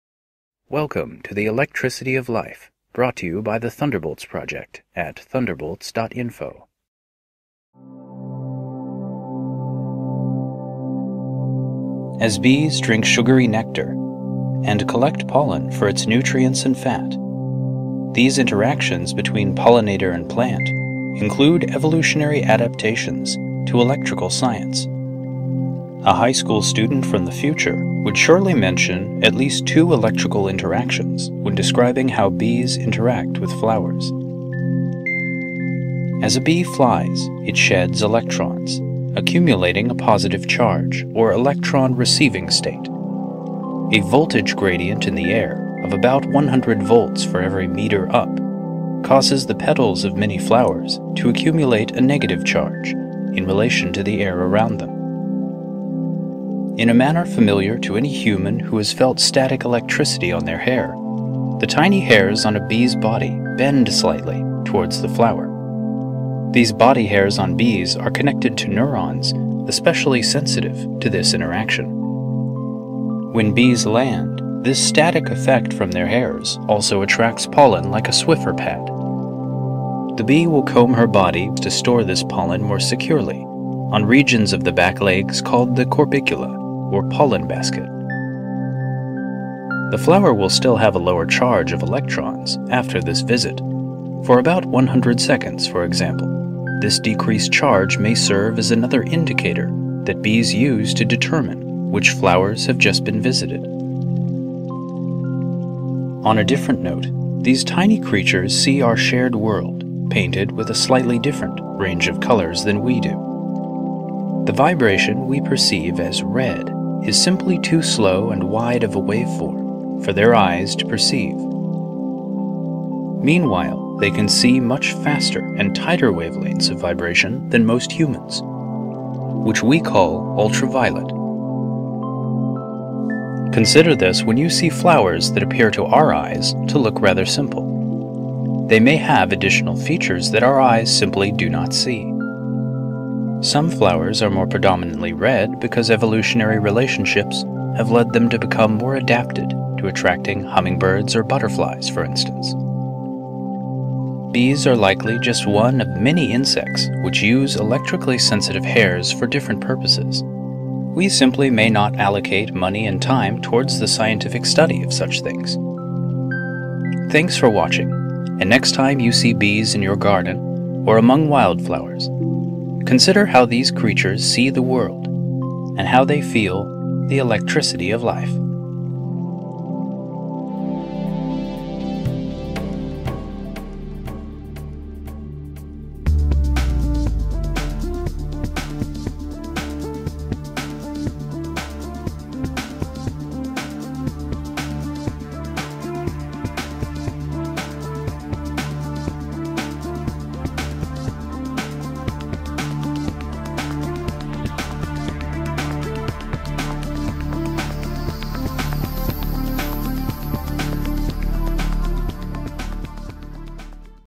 Bees and Electric Charge | Electricity of Life
An easy-to-follow video by Alex Fournier discussing new views from modern biology of the electrical interactions between bees and flowers. For both the pollen-harvesting process and the efficiency of foraging behavior, static electricity makes a bee’s life easier.

Primary scientific sources for this finding in popular science media:

Mechanosensory hairs in bumblebees detect weak electric fields.

Detection and learning of floral electric fields by bumblebees.